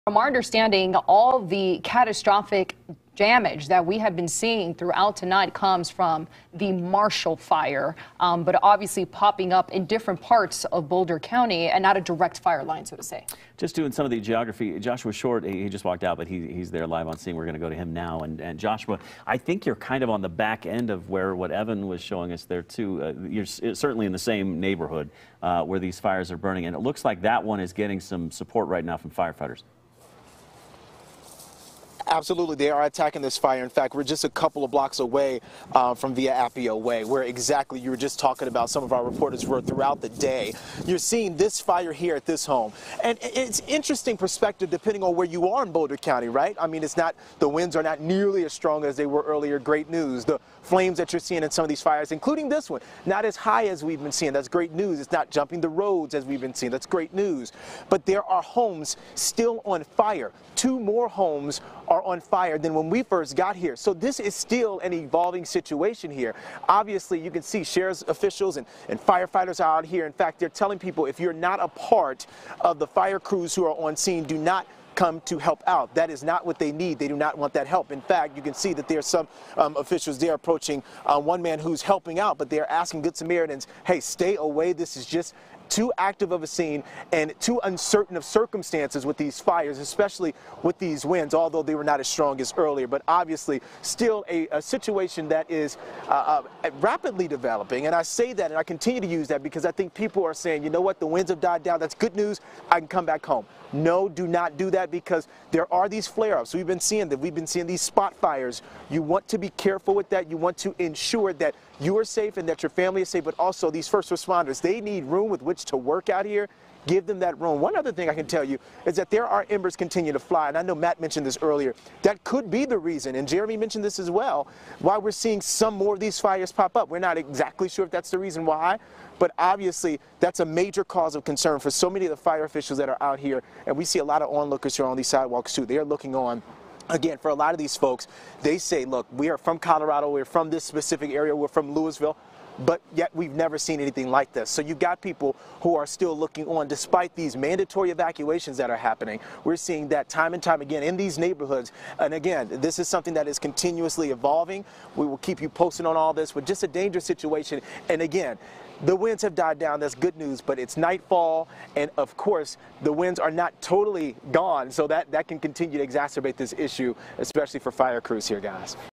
Firefighters respond to home near Via Appia Way
Joshua Short reports from the area of Via Appia Way, where firefighters were responding to fast-moving wildfires in the area.